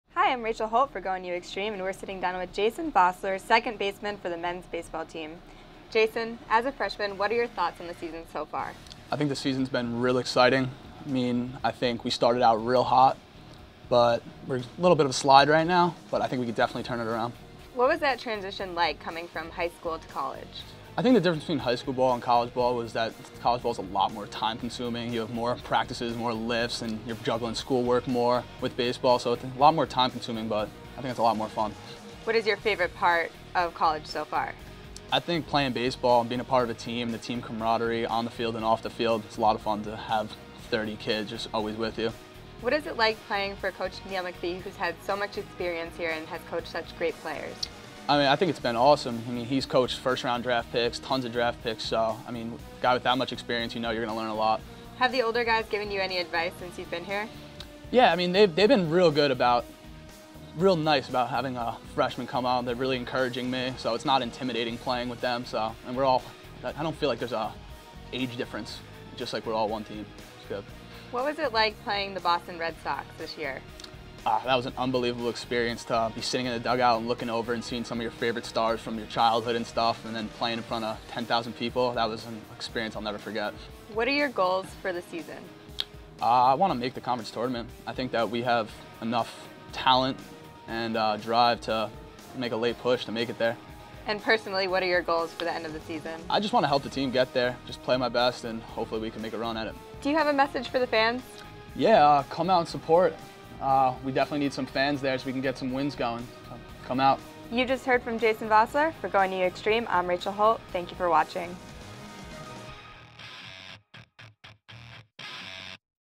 Get To Know Your Huskies - Jason Vosler
Rachel Holt of GoNUxstream sits down with Freshman Second Baseman Jason Vosler of the Northeastern University Baseball Team.